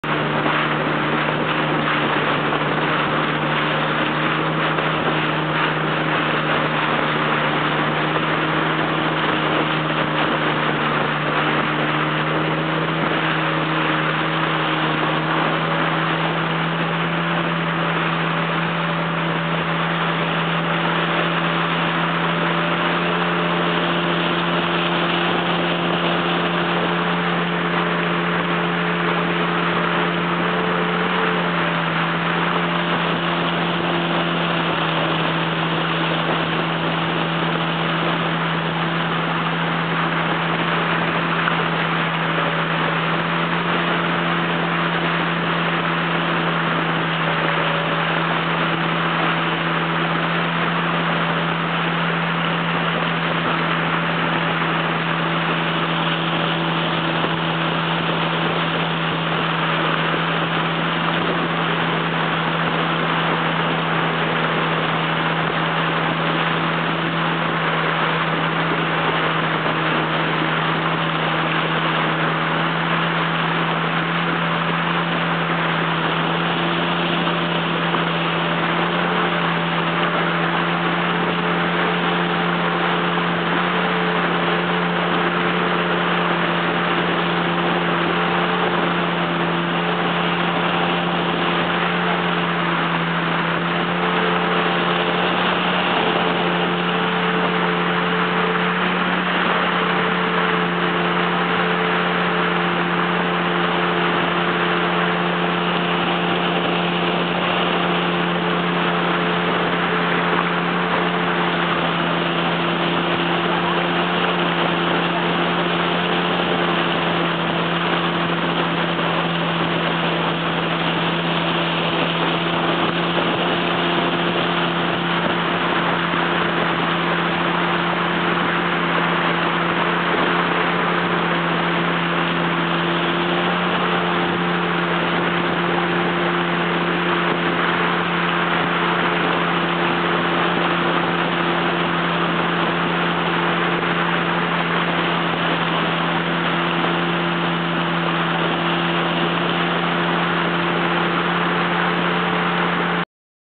El Rio Huayabamba en Setiembre de 2012
Navegando el Rio Huayabamba, en la Provincia de Mariscal Cáceres, en la ruta al caserío de Marisol.